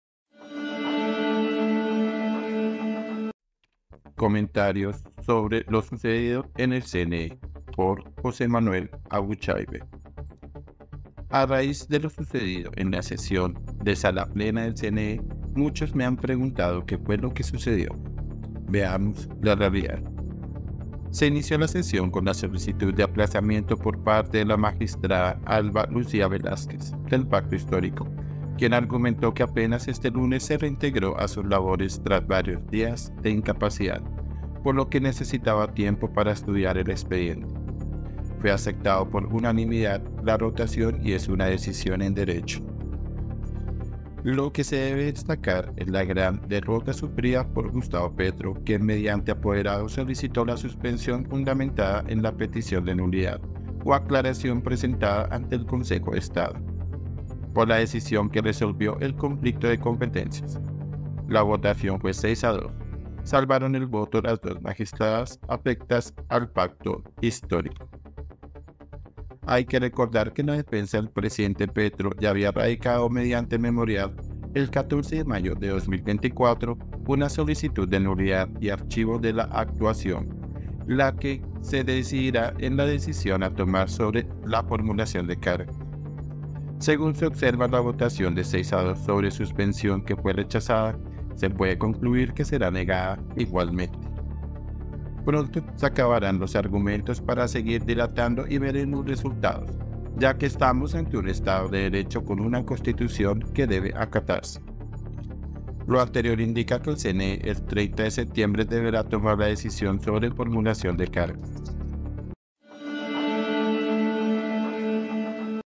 GRAN DERROTA SUFRIDA POR GUSTAVO PETRO 📢 Comentarios sobre lo sucedido en el CNE
Lo que se debe destacar es la gran DERROTA sufrida por Gustavo Petro, quien mediante apoderado solicitó la suspensión fundamentada en la petición de nulidad o aclaración presentada ante el Consejo de Estado, por la decisión que resolvió el conflicto de competencias. LA VOTACIÓN FUE 6 A 2. Salvaron el voto las dos Magistradas afectas al PACTO HISTÓRICO.
columna JOSÉ MANUEL ABUCHAIBE